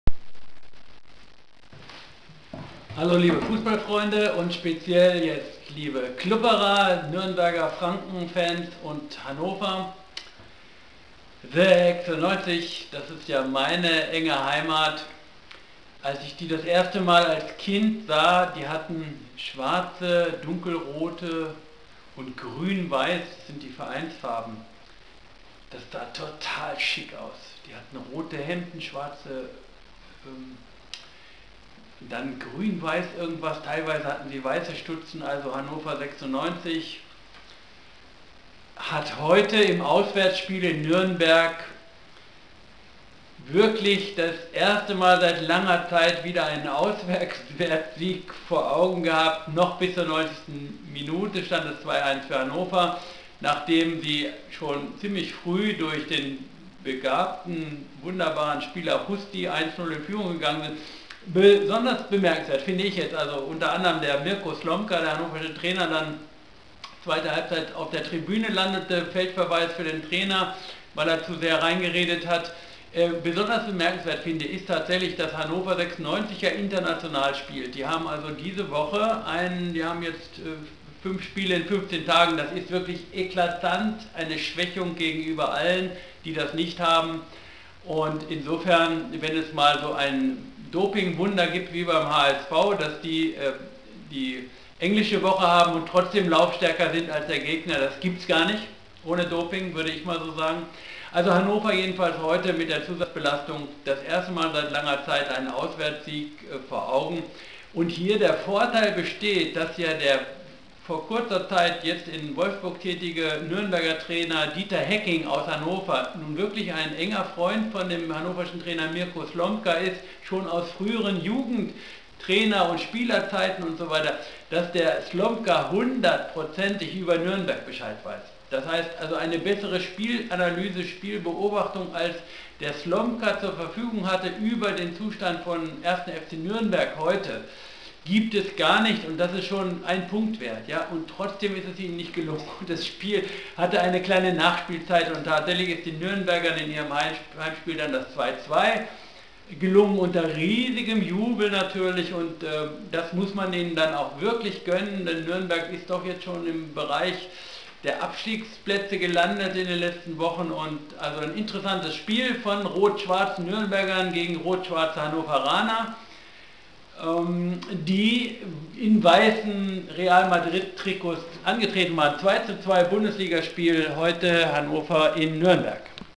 1. FC Nürnberg vs. Hannover 96 2:2 in Lichtgeschwindigkeit 2914
1. FC Nürnberg vs. Hannover 96 2:2 in Lichtgeschwindigkeit 2914 - Sonntag, 17. Februar 2013 Einzelheiten zur Lage - live und ungeschnitten -- aus Berlin
Thema: Den lange fälligen Auswärtssieg konnten die 96ger noch nach 90 Minuten erhoffen. Doch dann glich Nürnberg noch zum 2:2 Endstand aus. Hier konnte der Spitzenverein 96 aus der internationalen Belastung (15 Tage 5 Spiele) sich nicht erheben. Auch das Insiderwissen von Trainer Mirko Slomka, der mit Nürnbergs Ex-Trainer Dieter hecking (jetzt Wolfsburg) eng kommuniziert, half schließlich nur zum Platzverweis des Trainers auf die Tribüne. Immerhin  ein Unentschieden ist für die Nürnberger in der Abstiegszone ein verdienter Punktgewinn zur großen Freude des Heimpublikums.

Produktion, Performance, Autor, Direktor: Dr. Dietmar Moews 2013; Aufnahmetechnik und Admin: Apo-Ippcrew Berlin